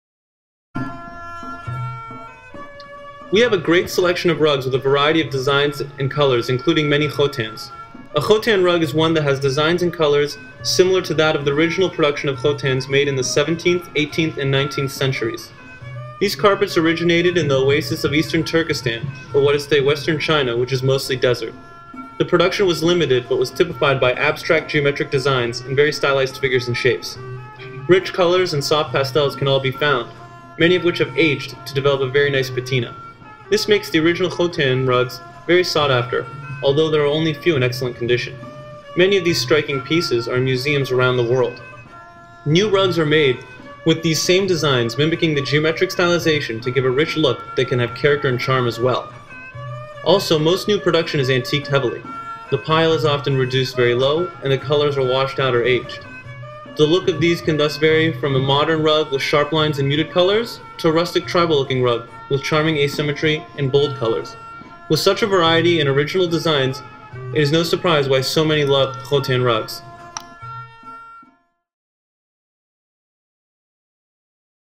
1800getarug | Khotan rugs
A Khotan rug is one that has designs and colors similar to that of the original production of Khotans made in the 17th, 18th, and 19th century. These carpets originated in the Oasis of Eastern Turkestan or what is today Western China which is mostly desert.